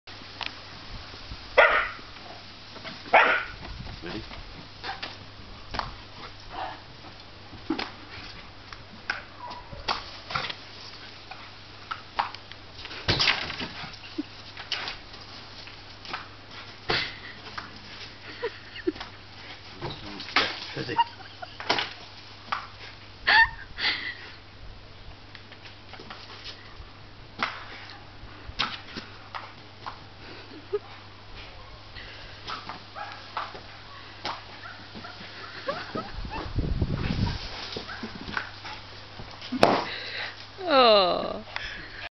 Doggy Balloon Keepy Uppy.AVI
Nine year old collie practising for David Beckhams replacement!!